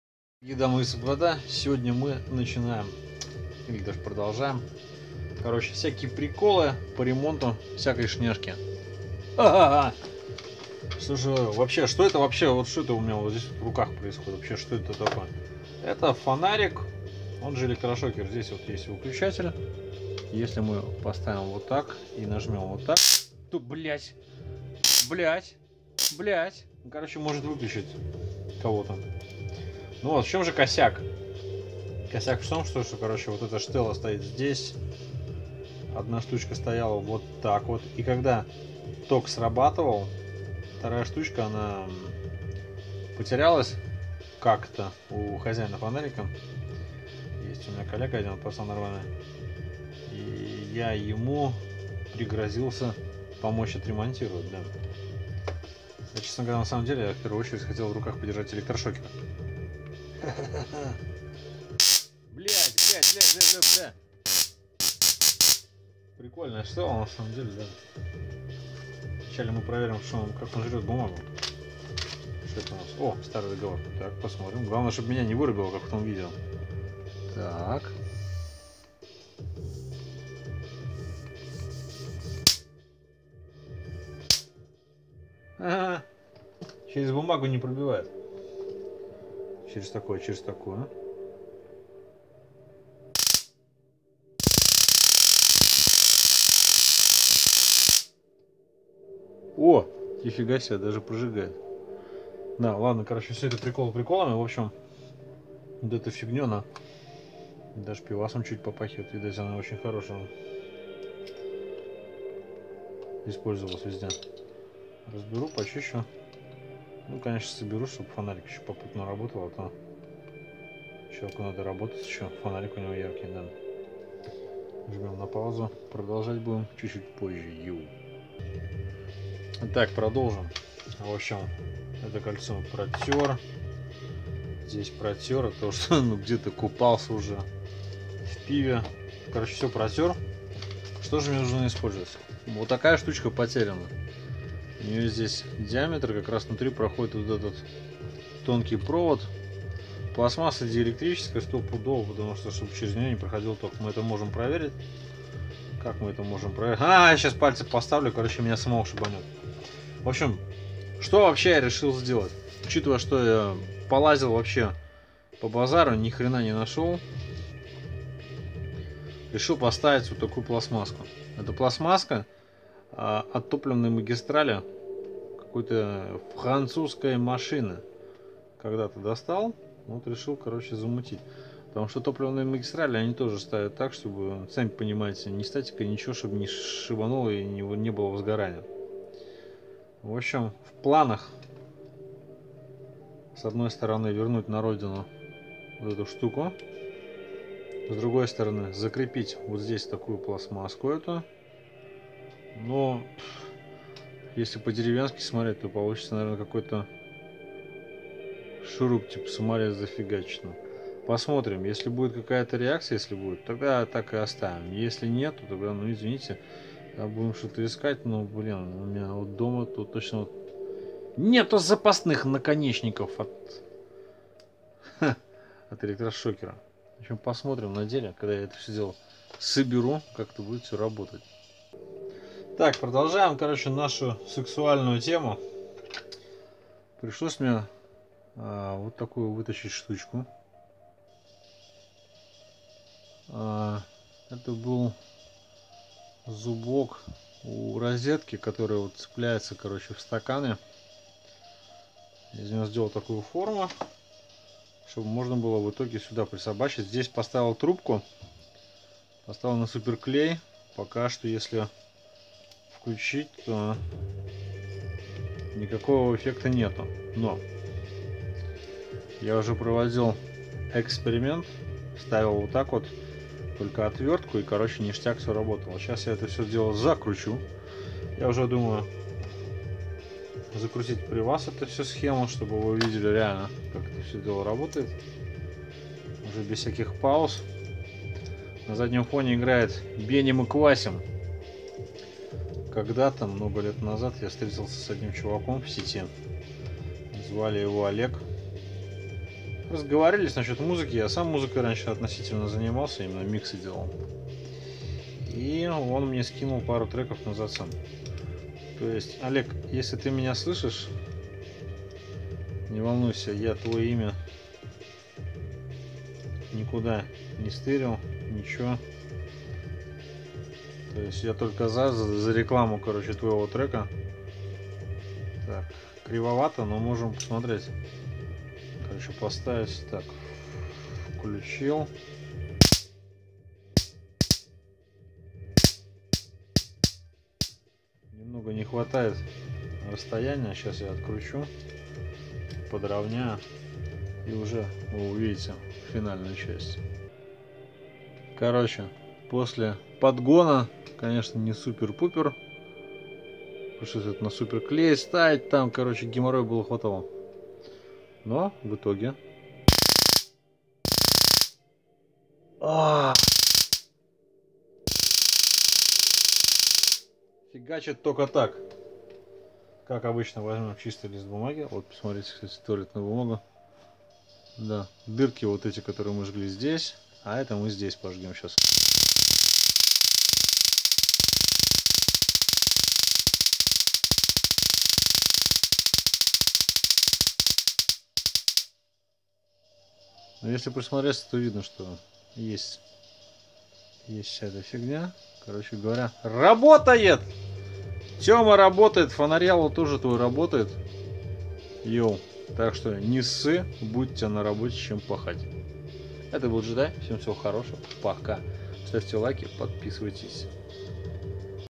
Реанимация электрошокера
Попал мне в руки электрошокер  с фонариков в одном флаконе, который слегка перестал работать. Решил отремонтировать и вот что из этого вышло